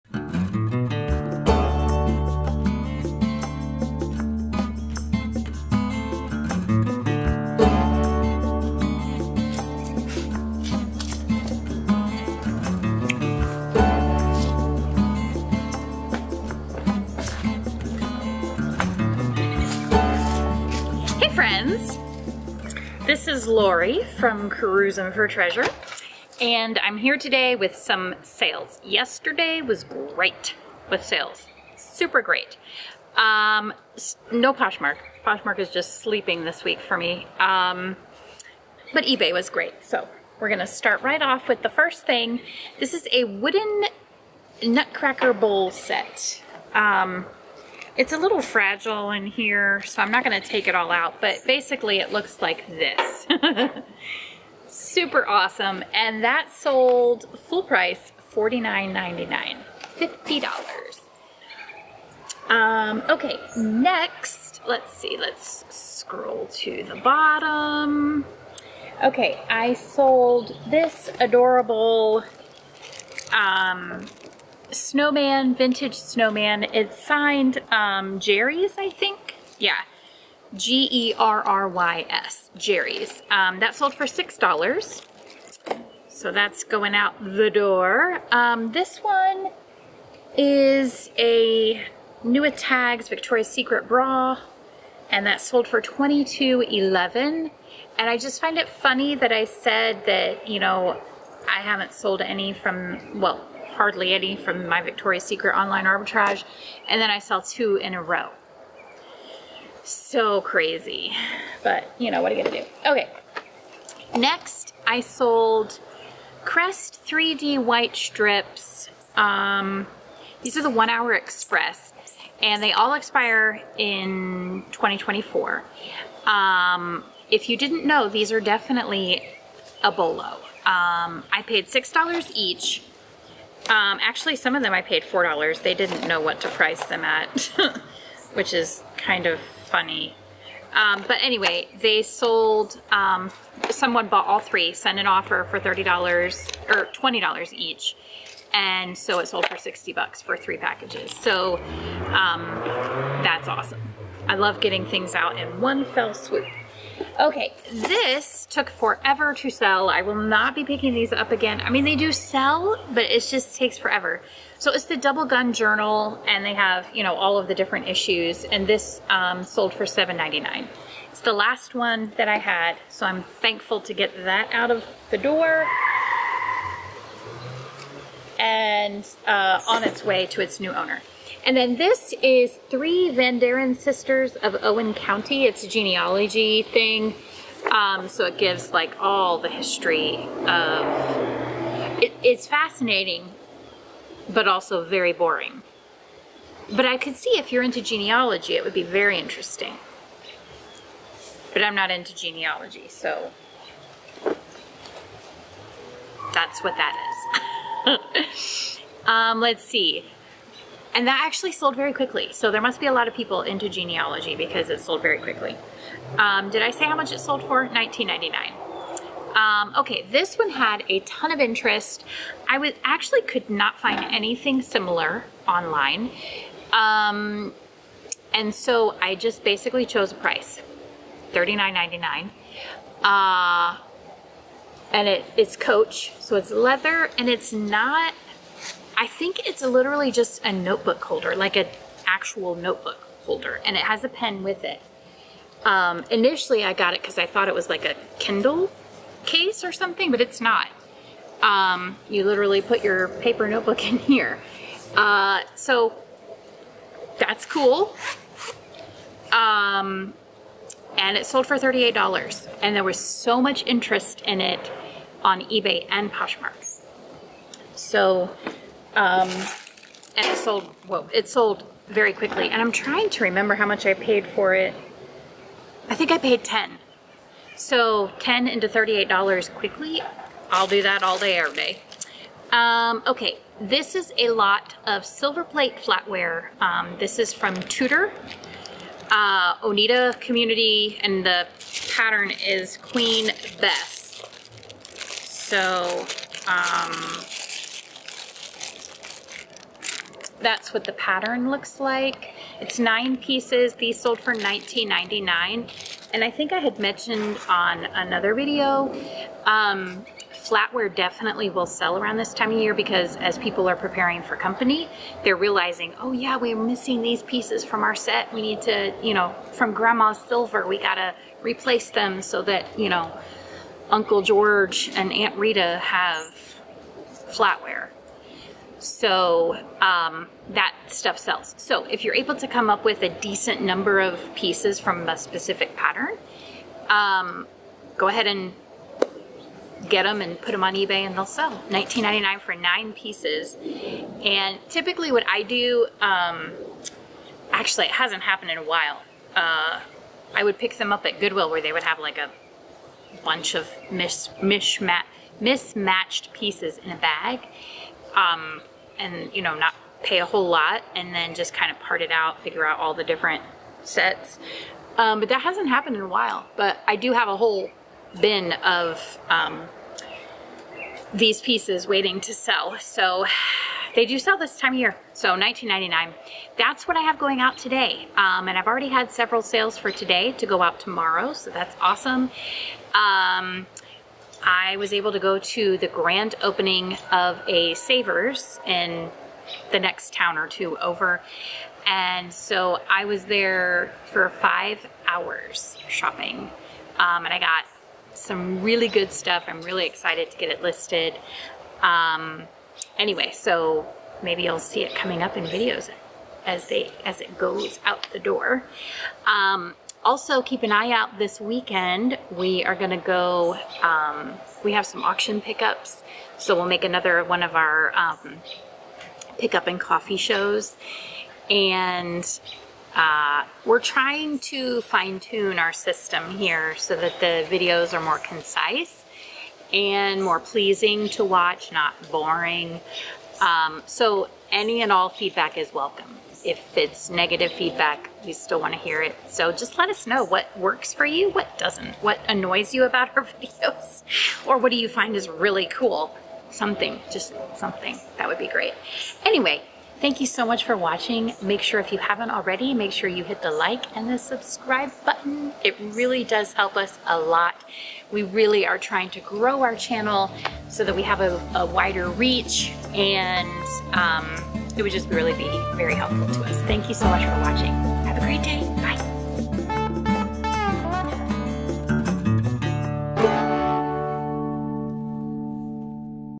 What Sold Show | Episode 13 | What sold for me on eBay | Mom Reseller Shares Her Sales
When your husband is your editor...🙄😂